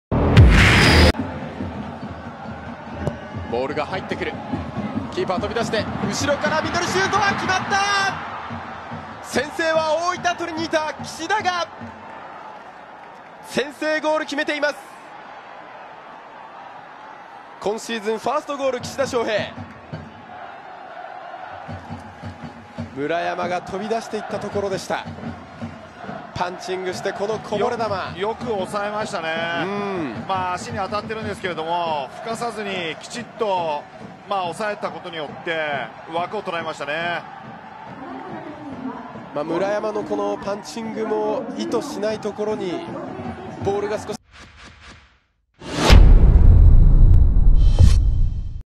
【公式】ゴール動画：岸田 翔平（大分）38分 松本山雅ＦＣvs大分トリニータ 明治安田生命Ｊ２リーグ 第38節 2017/10/21
CKのチャンスから相手GKが弾いたボールを岸田 翔平（大分）がダイレクトで叩き込み、大分が敵地で先手を奪う！明治安田Ｊ２のゴール動画を公開中！
2017年10月21日（土）に行われた明治安田生命Ｊ２リーグ 第38節 松本vs大分のゴール動画です。